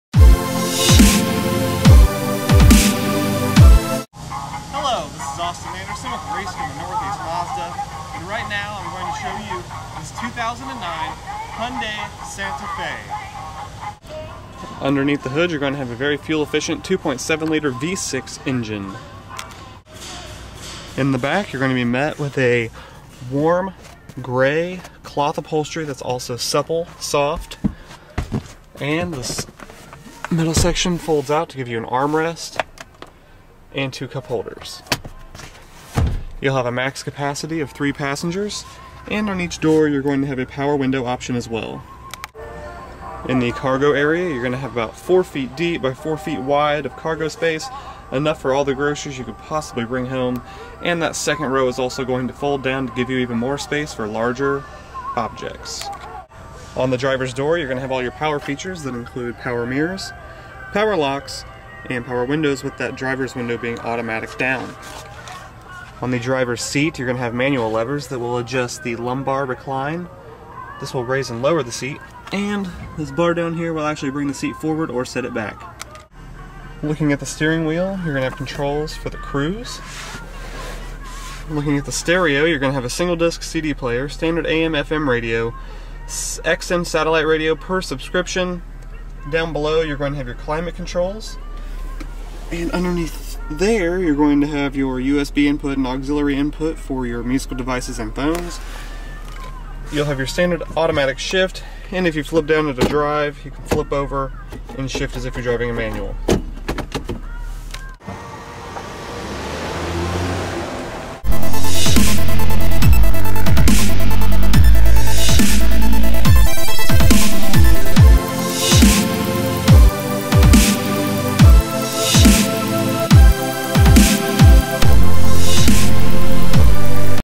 2009 Hyundai Santa Fe Walkaround Video
This 1-owner,2009 Hyundai Santa Fe is a fantastic family vehicle that offers not only the cargo space and leg room to aid your family, but it also contains awesome interior features that include a single disc cd player, auxilary audio input, USB input, cruise control, power locks/mirrors/windows, and satellite radio!